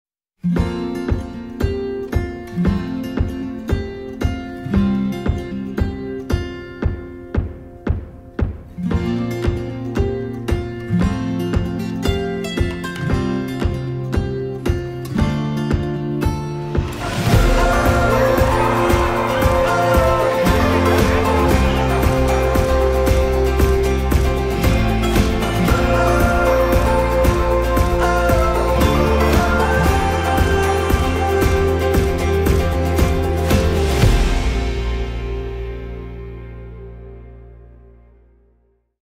Spring | Springfield Manor Winery | Elegant Thurmont, MD Wedding Venue!
TLIC Media had a wonderful time filming Quintin & Daisy's wedding celebration at Springfield Manor Winery, Distillery, and Brewery! Located in Frederick, Maryland, this wedding venue is filled with historic charm from the exterior design and interior chandeliers, to the dance floor inside the rustic barn! Known for the lovely scented air from the nearby lavender fields, this romantic wedding venue offers a breathtaking view of the Catoctin Mountains sure to make your special day feel like something out of a fairytale!